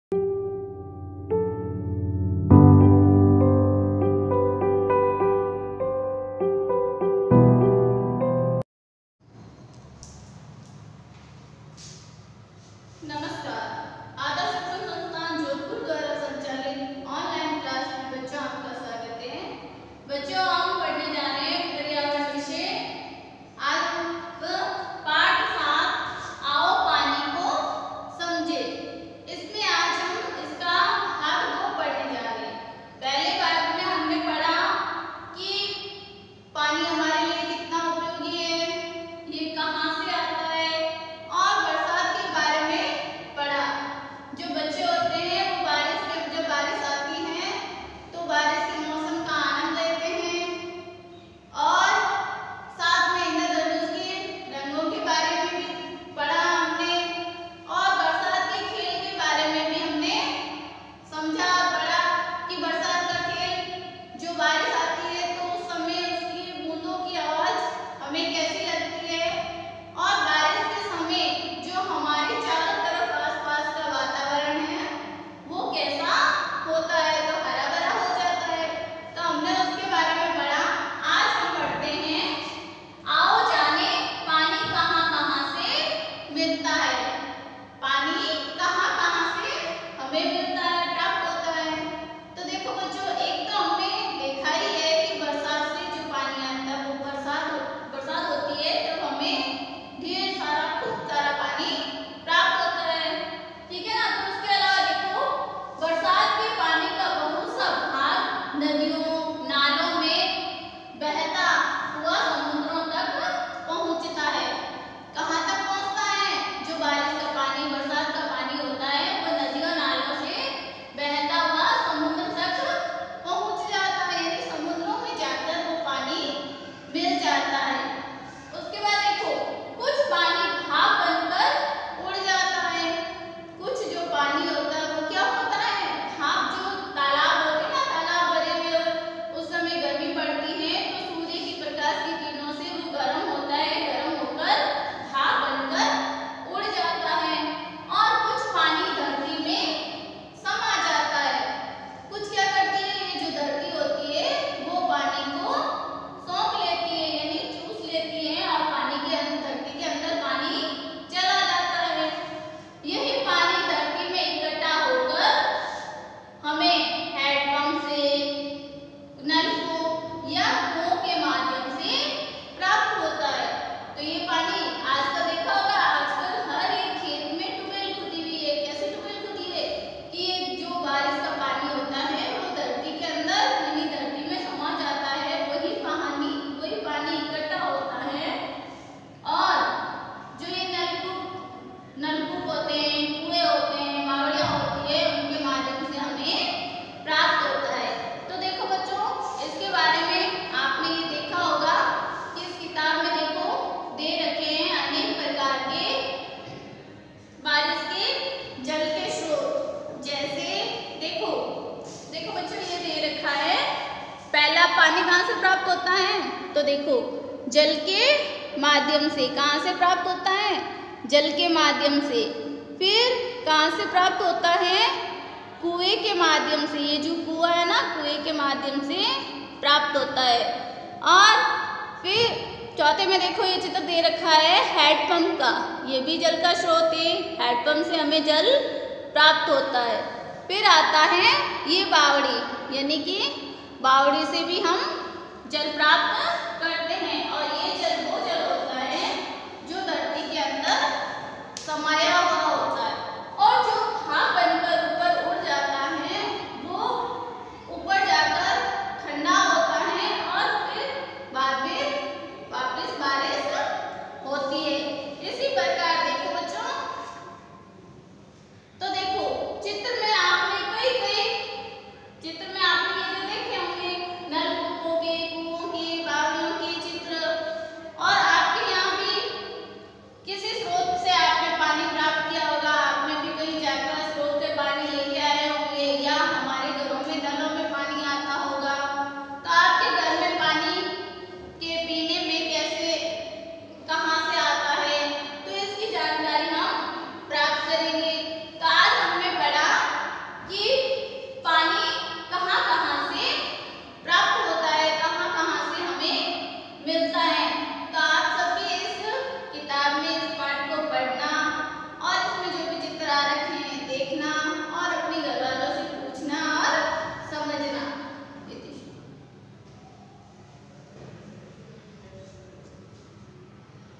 कक्षा तृतीया, विषय पर्यावरण। कालांश 22, पाठ 7, आओ पानी को जाने। प्रकरण- पानी कहां कहां से मिलता है।
Video from Bhanwar Lal Suthar